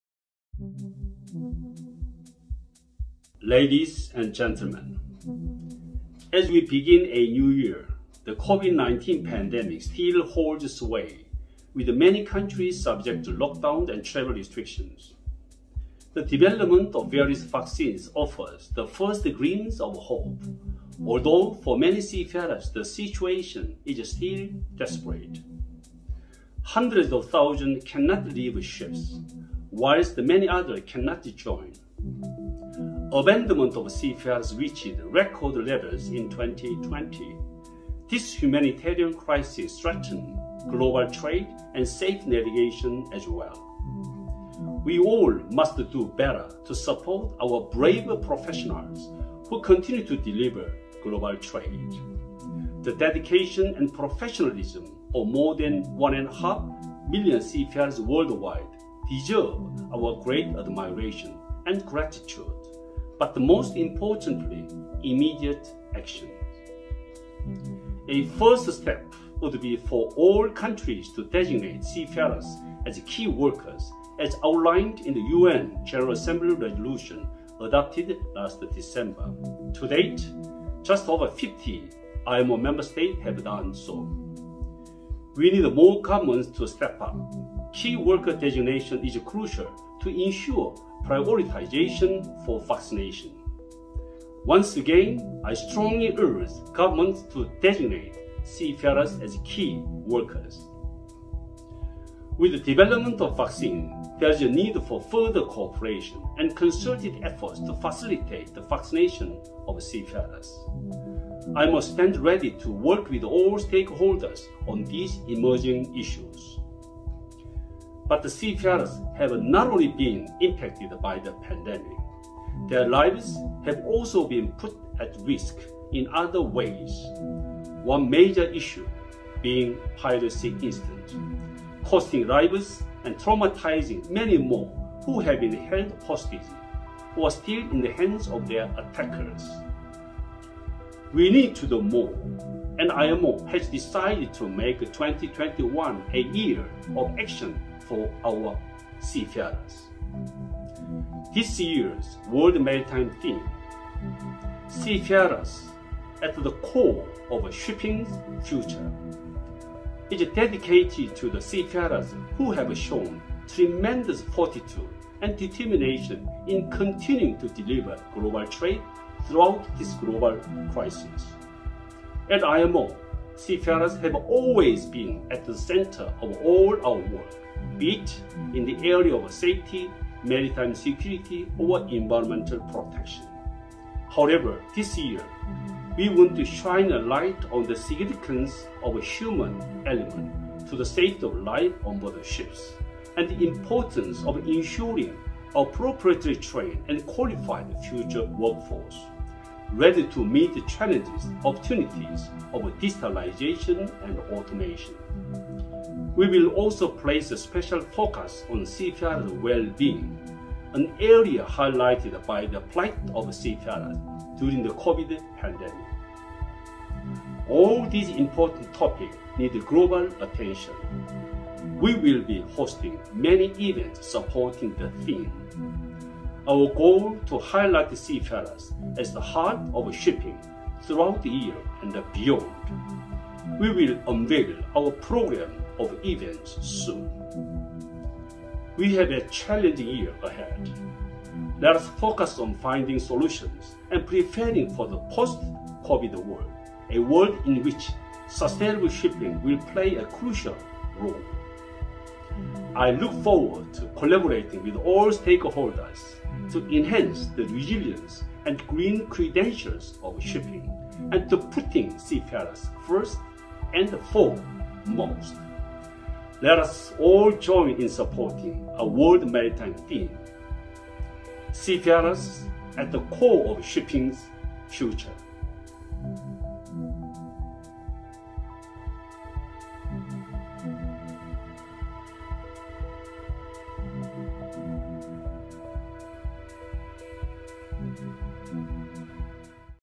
World Maritime Day 2021 - IMO Secretary General's message
Video message by IMO's Kitack Lim on this year's World Maritime theme: "Seafarers: at the core of shippingʹs future" reflecting a clear need to raise awareness of seafarersʹ vital role in world trade and increase their visibility.
The focus on seafarers comes as the COVID-19 pandemic has placed extraordinary and unprecedented demands on seafarers.